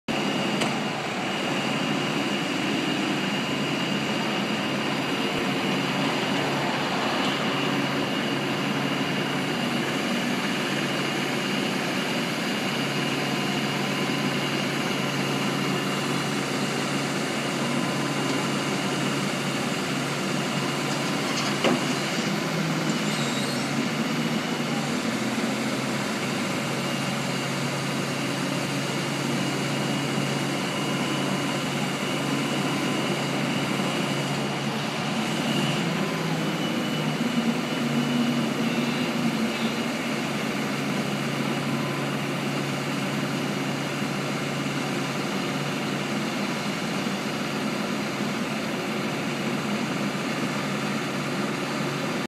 Escalator Installation
A crane truck lifts an escalator off of a flatbed truck for installation in a remodeled shopping center.